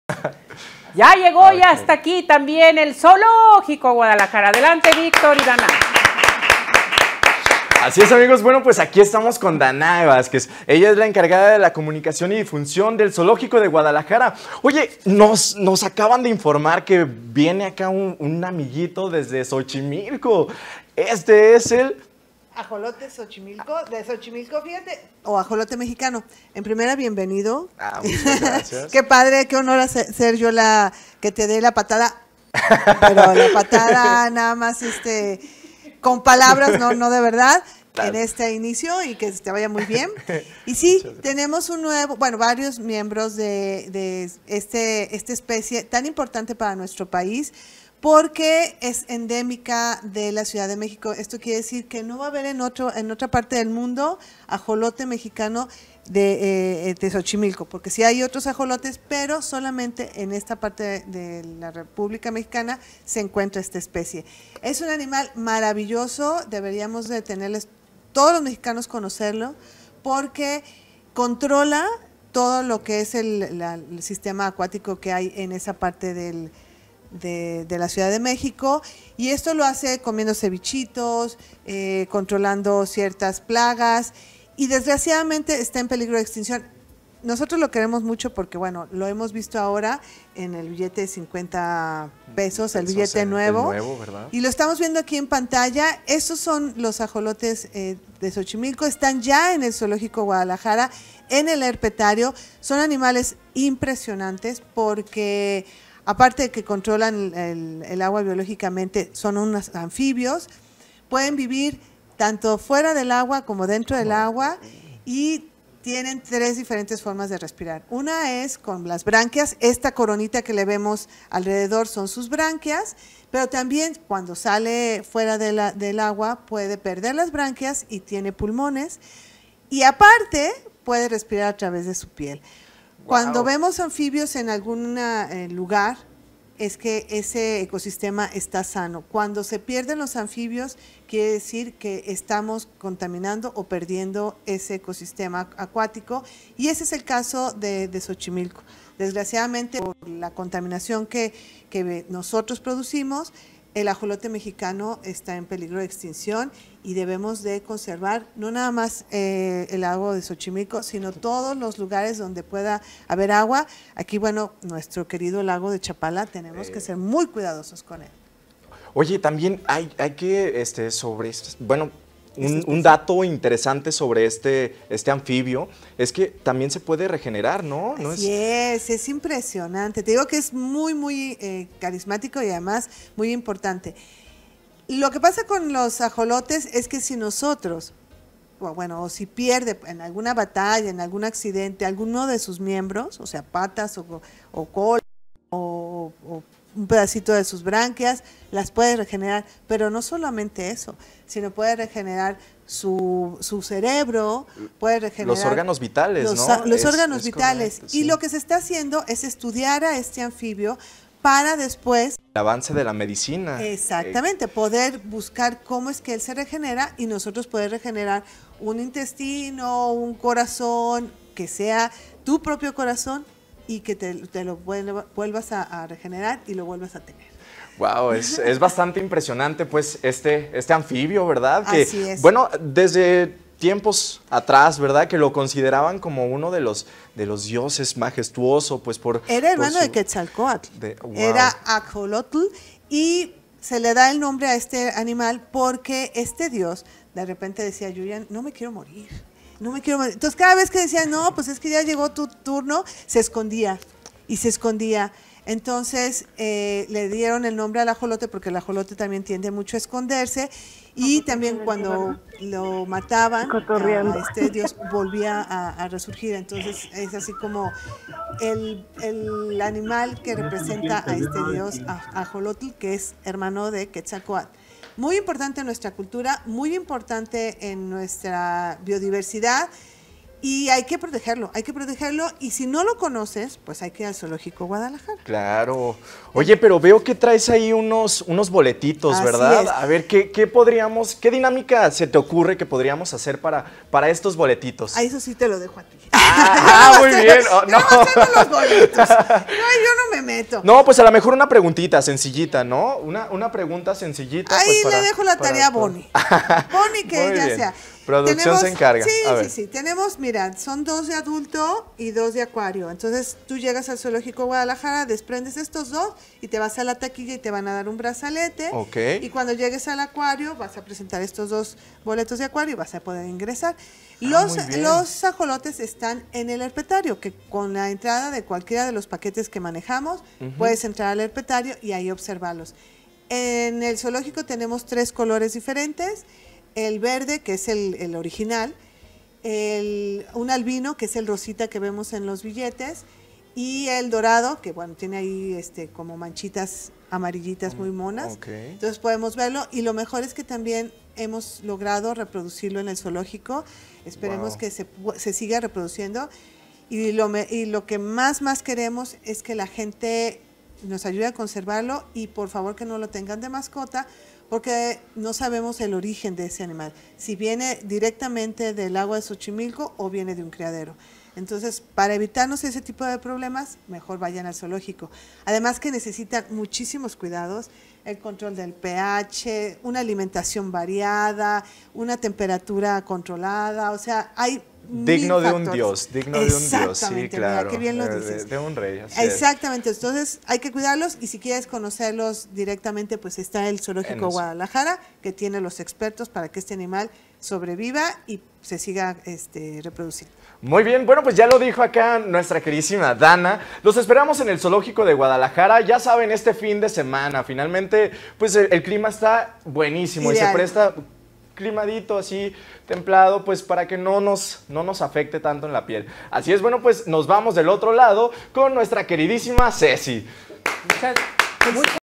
Zoológico | ¡Llegó el Axolote de Xolochimilco al Zoológico Guadalajara 🤩 !
¡Llegó el Axolote de Xolochimilco al Zoológico Guadalajara 🤩 !

🦎 La Lic. Danaé Vázquez, Jefa de Comunicación del Zoológico Guadalajara, nos platica todo sobre la llegada del nuevo inquilino.